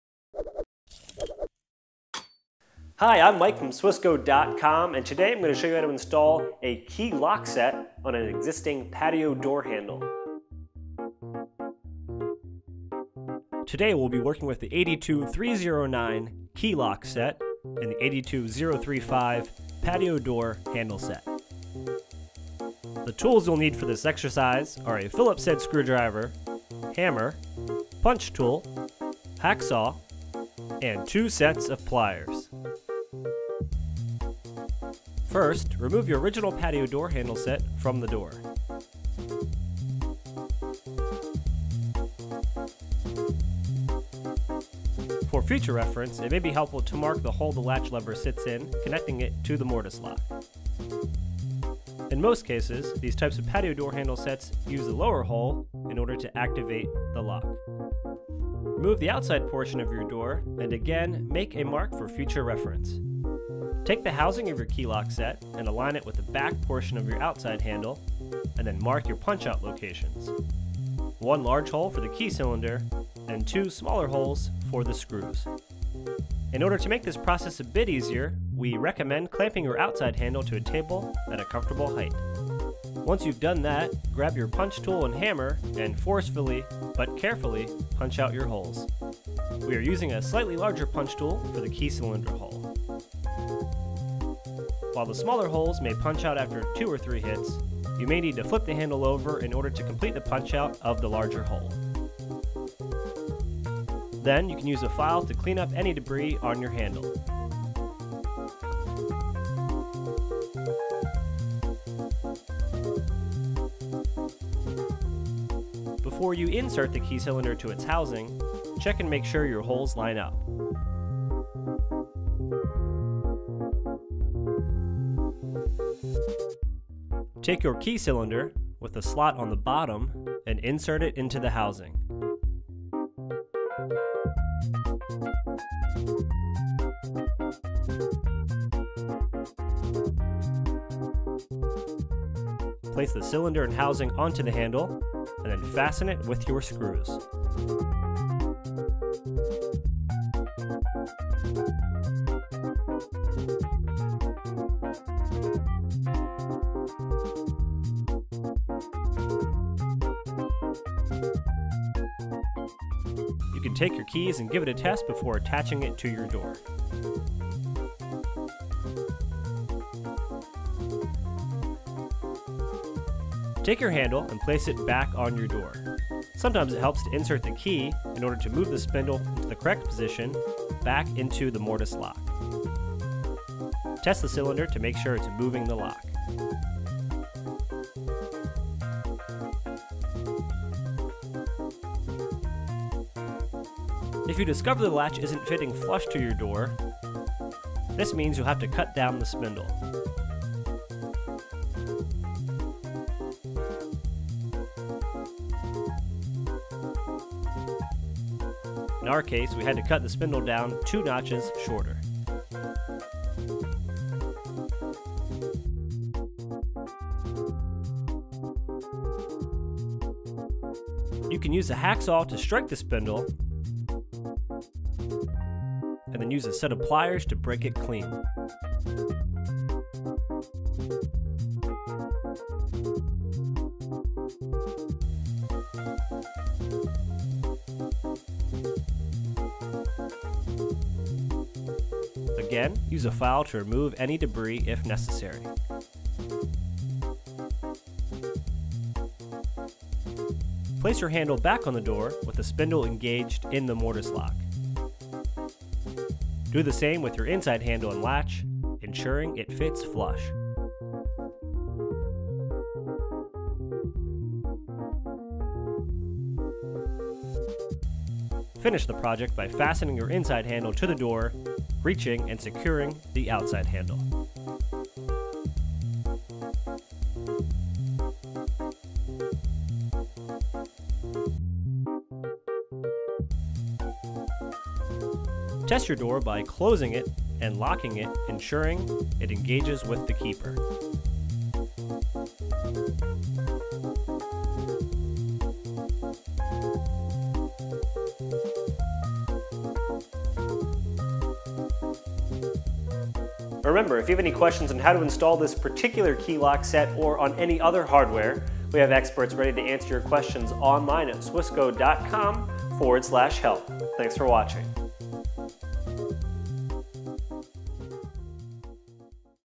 How to Install a Key Lock on a Patio Door [1080p]
In this video we show you how to install a key lock set on an existing patio door handle.

Products used in this video:

Key Lock and Housing
Product Code: 82-315

Patio Door Handle Set, 3-15/16"
Product Code: 82-035

Adjustable Hacksaw
Product Code: 75-023

12" Hacksaw Blades (5 Pack)
Product Code: 75-023B

Locking Long Nose Pliers
Product Code: 75-054

Long Nose Pliers
Product Code: 75-055

0:00 Intro
0:13 Parts
0:22 Tools
0:34 Removing the handles
1:16 Punching out holes
1:54 Assembling the key cylinder
2:53 Testing the handles and lever
3:13 Cutting the spindle to length
4:10 Completing the installation